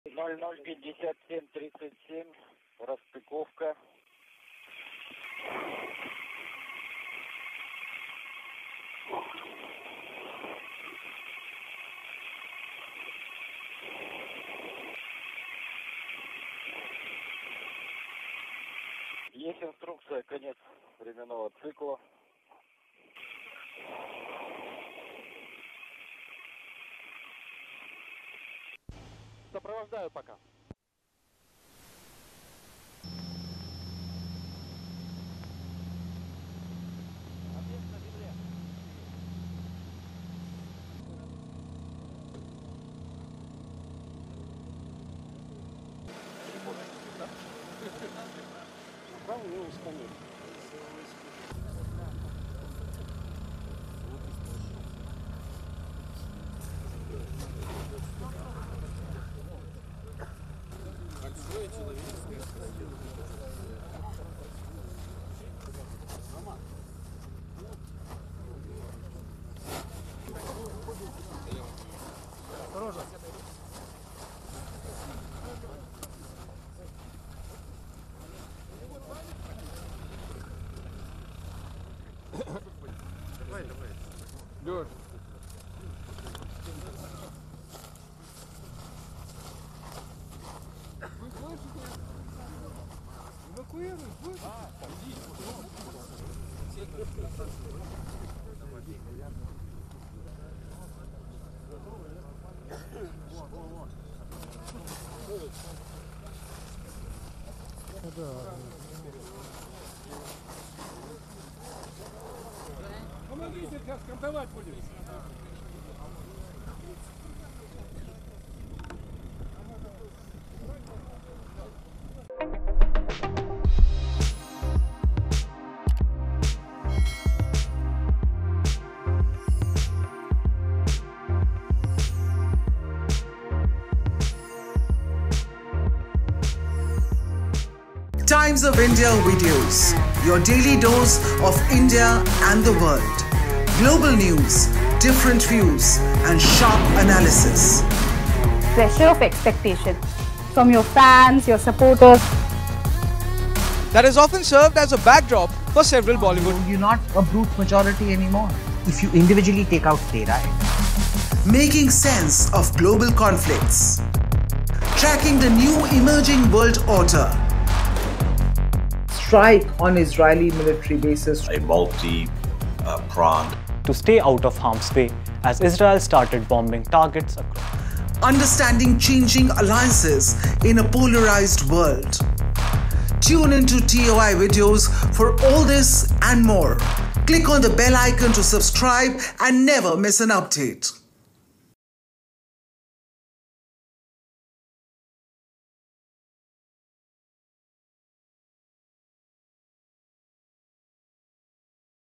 RARE: Russian & American Astronauts' Epic Return To Earth Together After 220 Days | Watch
Russia’s Soyuz MS-26 spacecraft has triumphantly returned to Earth, completing a high-stakes 220-day mission with cosmonauts Alexey Ovchinin, Ivan Vagner, and NASA veteran Donald Pettit aboard. The trio safely landed in Kazakhstan after orbiting Earth 3,520 times and covering 150 million km in space.